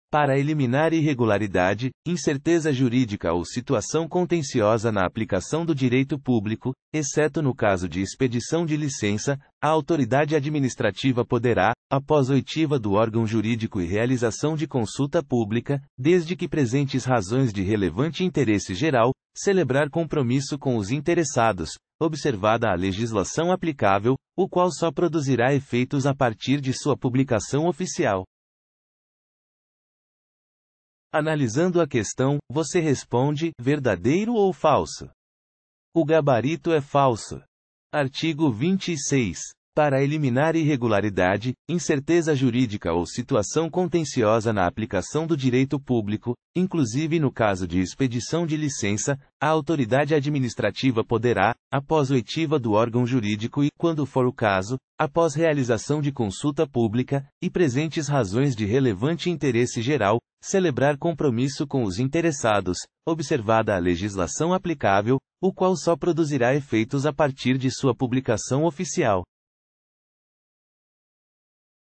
QScon Direito: [Art. 26, LINDB] FAUEL 2019 - Assessor Jurídico - Prefeitura de Jandaia (PR)
Art. 26, LINDB | FAUEL 2019 | Assessor Jurídico Prefeitura de Jandaia | (PR)

Para eliminar irregularidade, incerteza jurídica ou situação contenciosa na aplicação do direito público, exceto no caso de expedição de licença, a autoridade administrativa poderá, após oitiva do órgão jurídico e realização de consulta pública, desde que presentes razões de relevante interesse geral, celebrar compromisso com os interessados, observada a legislação aplicável, o qual só produzirá efeitos a partir de sua publicação oficial.

Gabarito: Falso